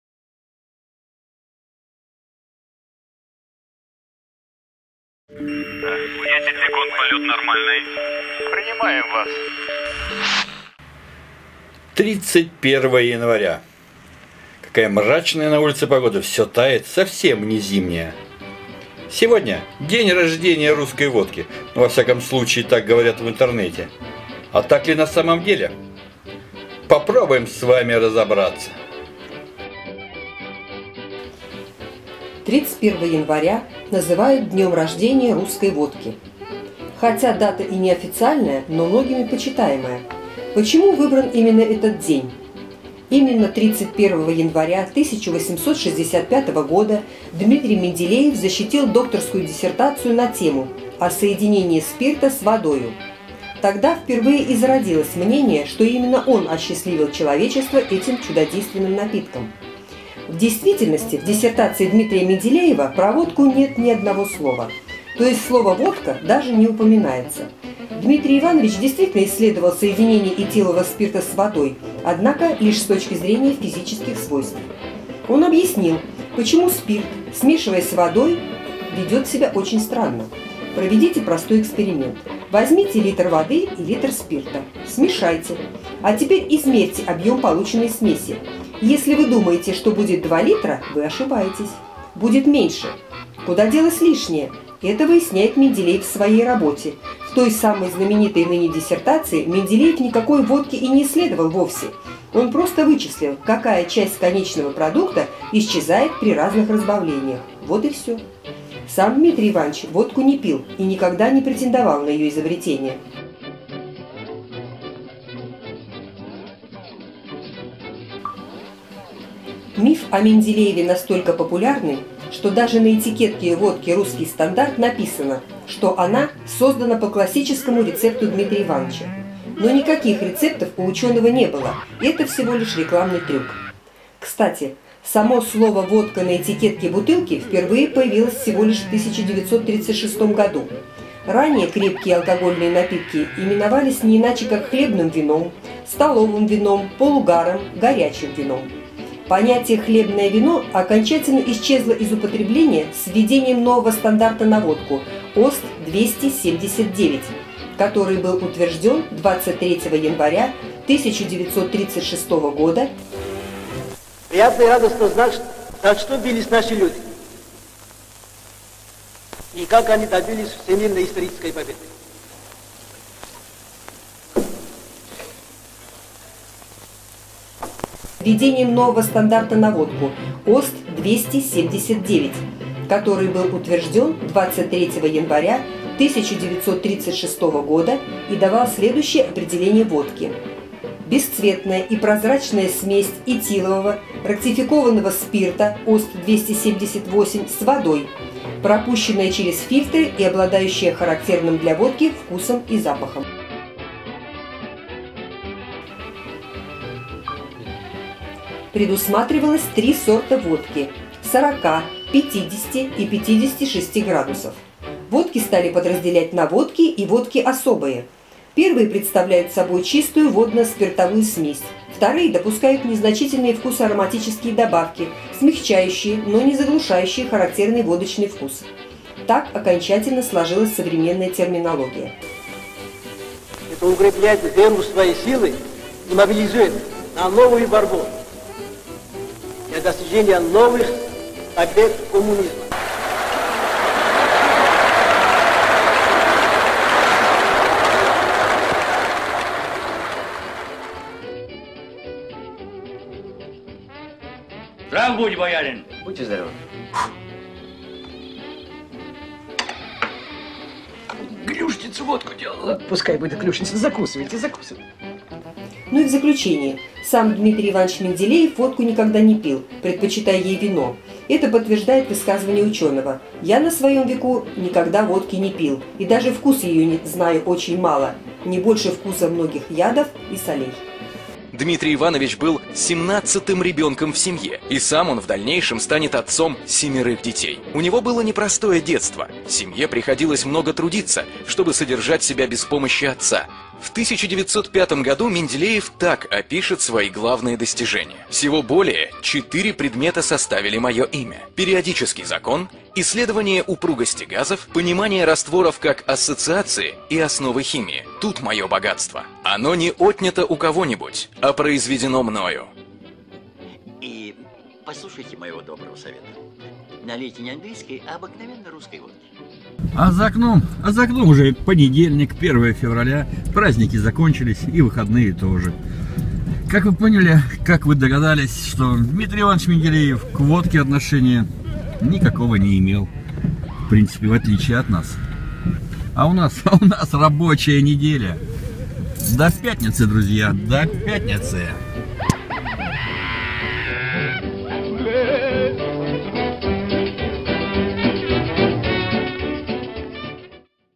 Кто придумал водку? Миф о Менделееве и водке.
31 января - День Российской водки! Кто придумал водку? Имеет ли к этому отношение Менделеев? Разберемся!?